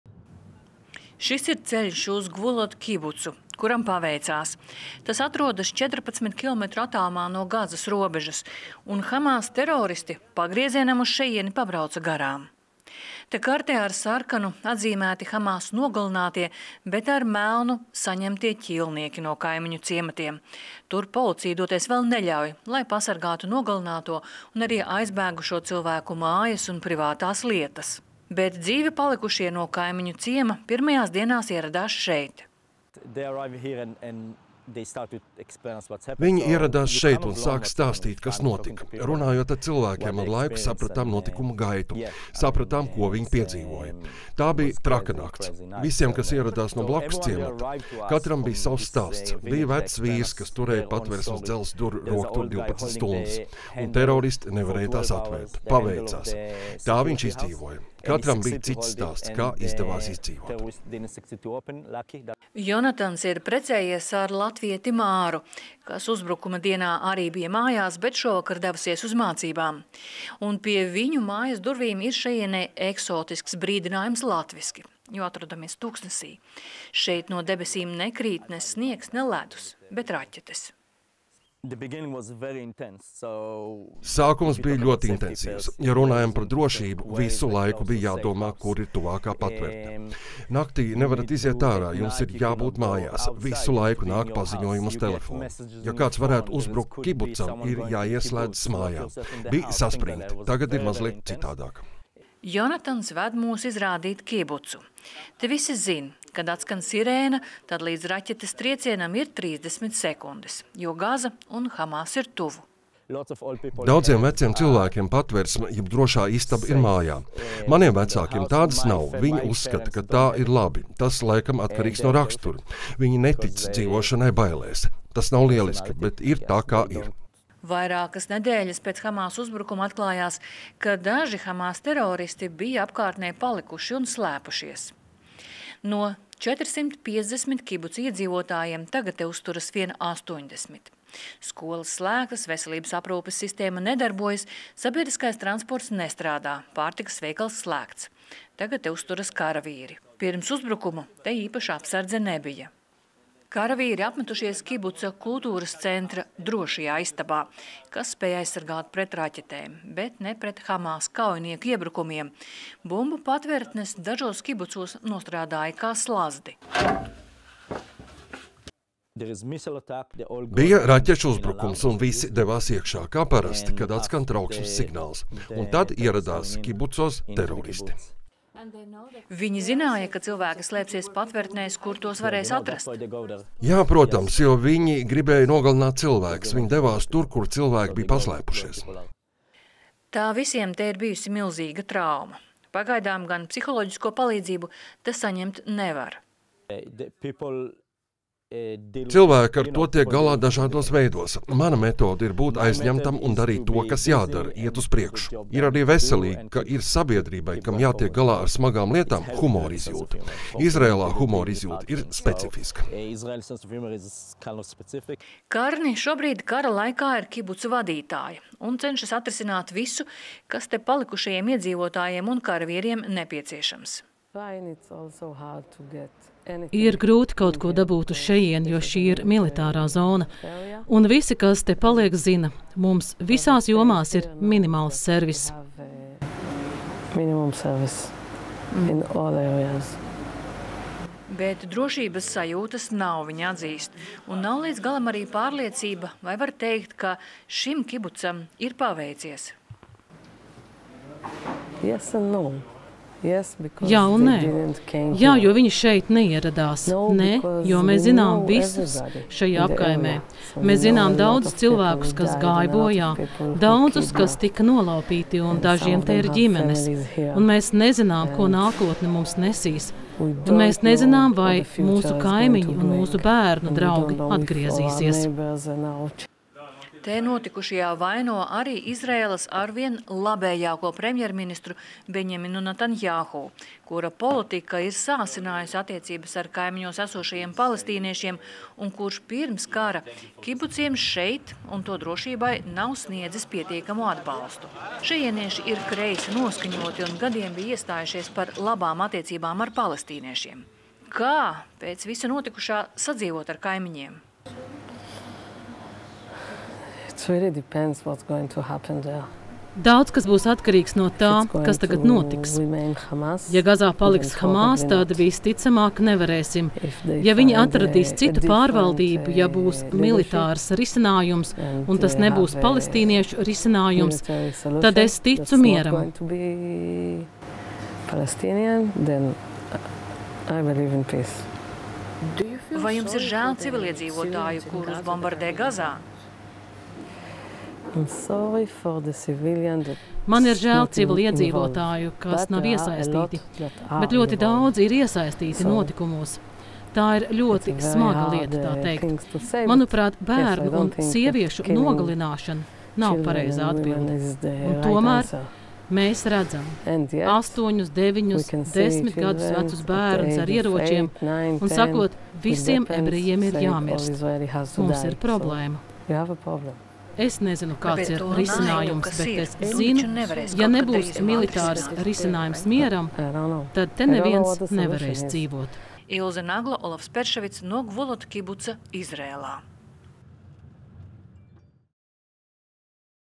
Reportāža no Gazas pierobežas: kibucs, kuram paveicās
Kibucs, kuram paveicās – tā var teikt par Gvulot kibucu Izraēlas dienvidos, 12 kilometrus no Gazas robežas. Par notikušo 7. oktobrī tur gan runā nelabprāt – jo viss notikušais vēl ir pārāk dzīvs un nekas vēl nav beidzies.